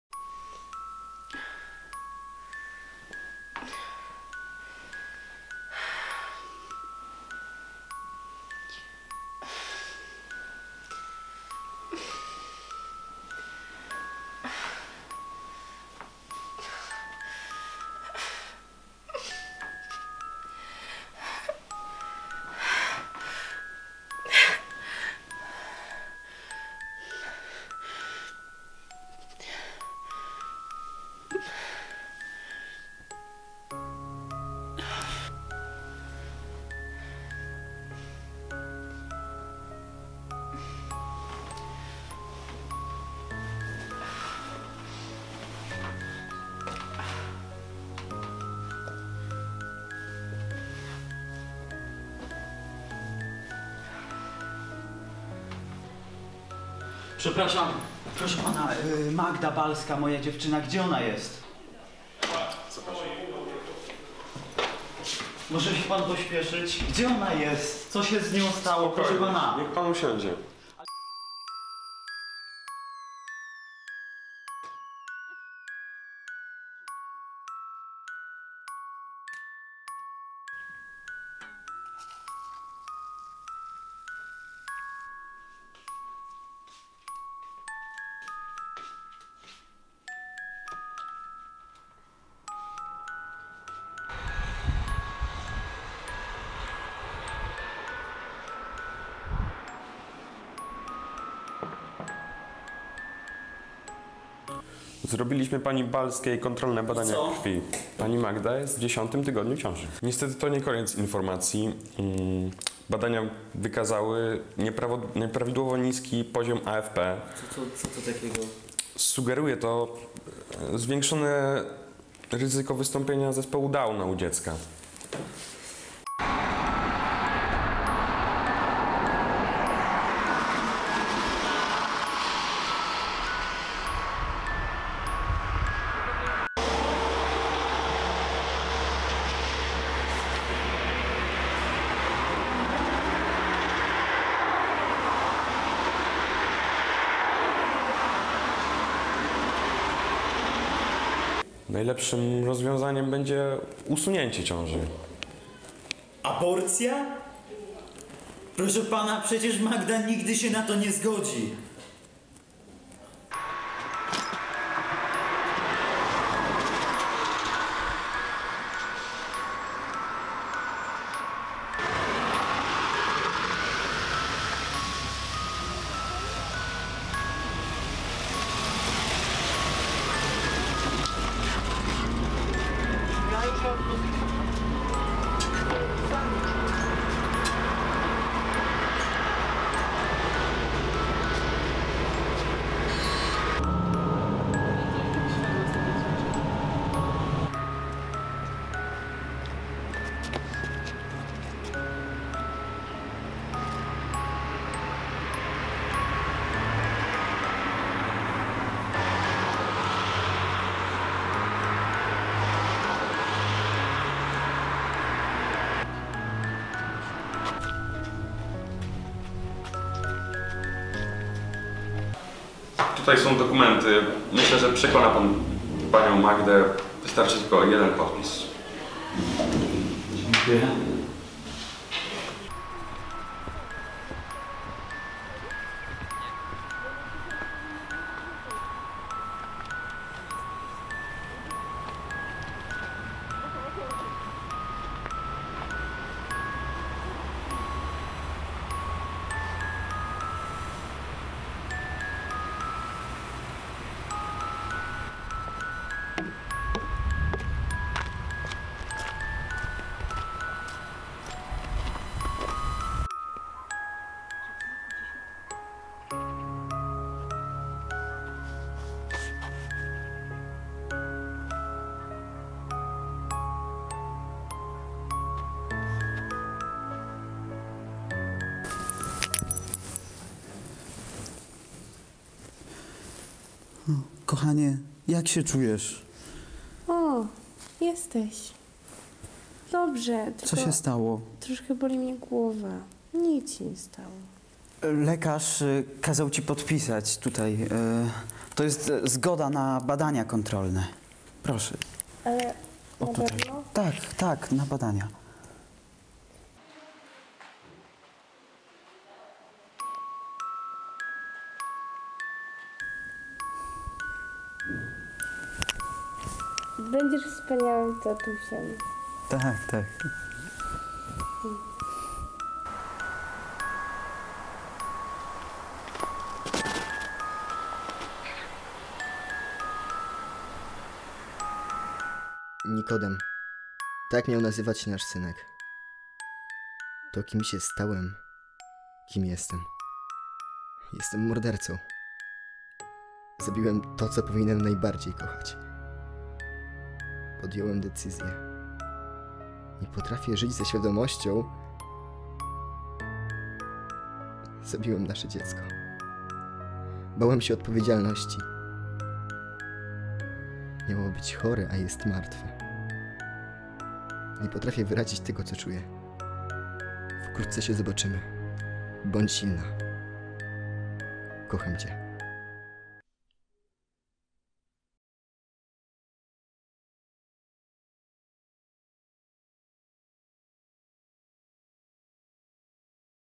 Beze mnie
XIV edycja Konkursu o nagrodę im. ks. Jerzego Popiełuszki dla młodzieży w wieku od 14 do 19 lat „Pomóż ocalić życie bezbronnemu", kategoria multimedialna, I nagroda: Marcel Rzemieniuk, Rzeszów – „Beze mnie”